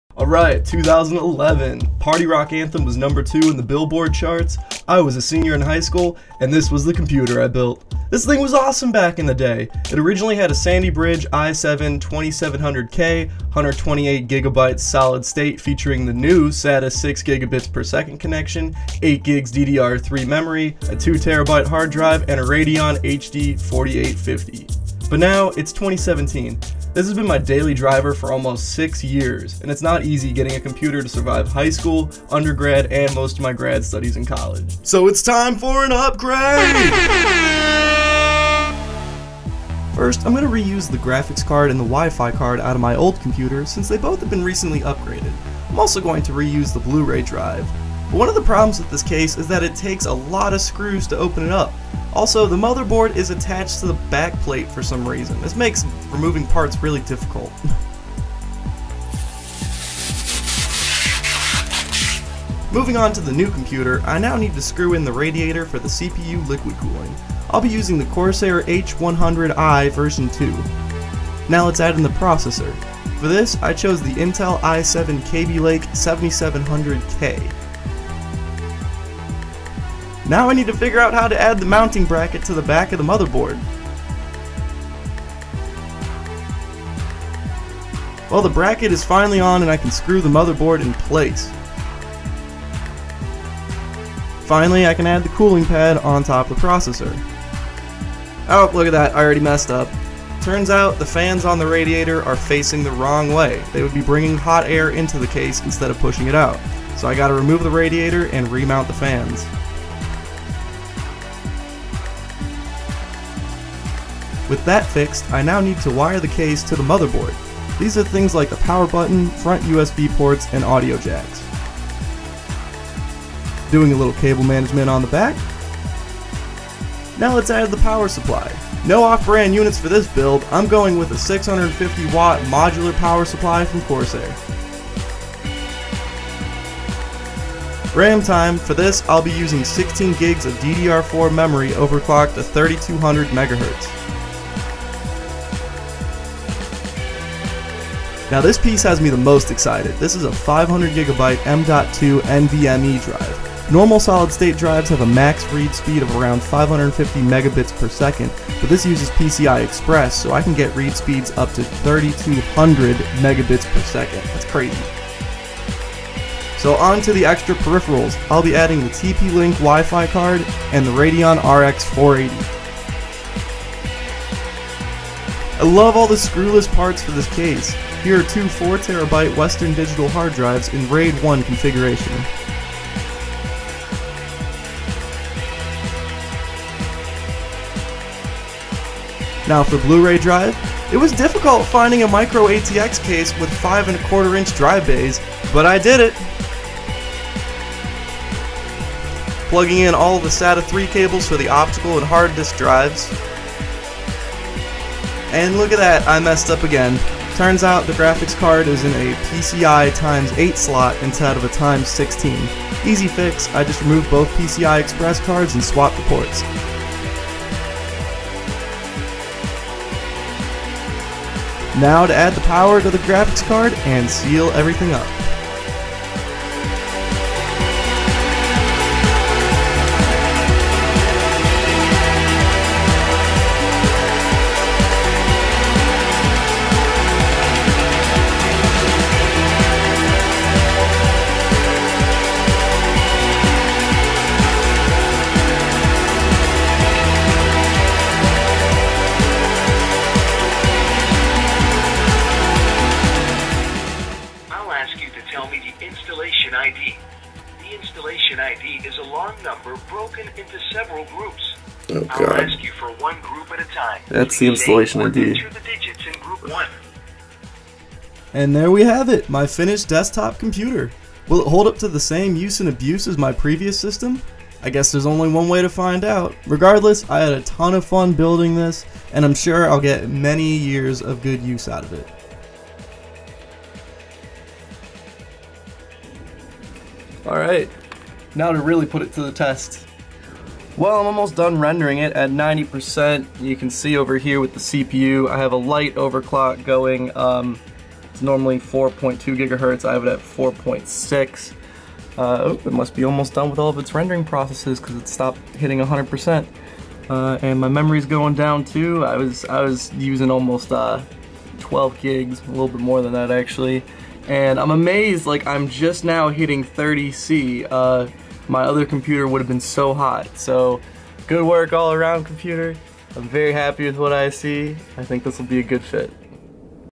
Desktop Computer 2017
With the recent release of Intel’s Kaby Lake processor, I figured it was finally time to upgrade my old computer!
Specs:
Processor: i7 Kaby Lake 7700K
GPU: Radeon RX480 8GB
Memory: DDR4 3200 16GB at 3200 MHz
Motherboard: Gigabyte GA-Z270MX-Gaming 5
HDD: Dual 4TB Western Digital Blue Hard Drives (Raid 1)
M.2: Samsung Evo 960 500GB Pci-e x4
PSU: Corsair CX G50M 650W PSU
Cooler: Corsair H100i v2